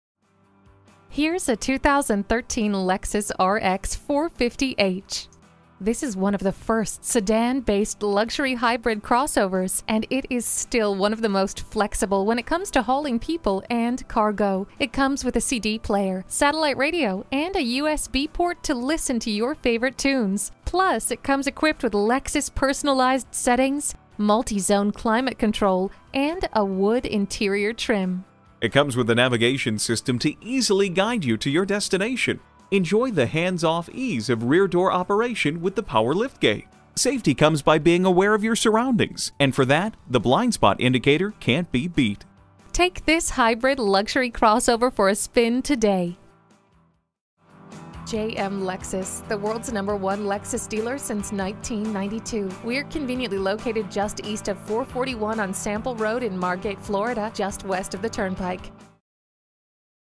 Certified 2013 Lexus RX 450h Margate FL Ft-Lauderdale, FL 561440A - SOLD
Year: 2013
Make: Lexus
Model: RX 450h
Trim: Premium w/Navigation
Engine: 3.5L V6 Hybrid
Transmission: CVT (Continuously Variable)
Color: Stargazer Black
Interior: Black
Mileage: 19276
Stock #: 561440A
VIN: JTJZB1BA9D2008442
!! 1 OWNER / CLEAN CARFAX !! LEXUS CERTIFIED! 3 YR/100K WARRANTY! BUY WITH CONFIDENCE! NAVIGATION SYSTEM!! LEATHER INTERIOR!! POWER MOONROOF!! PARKING SENSORS!! REAR BACKUP CAMERA!! BLUETOOTH!! Want to save some money? Get the NEW look for the used price on this one owner vehicle. Previous owner purchased it brand new! Lexus Certified Pre-Owned means you not only get the reassurance of a 3yr/100000 mile limited warranty but also a 161-point inspection/reconditioning 24/7 roadside assistance trip-interruption services loaner car complimentary oil/filter change and a complete CARFAX vehicle history report. This RX has only been gently used and has low low mileage on its gas-saving Hybrid engine. They don't come much fresher than this!

Address:
5350 W Sample Rd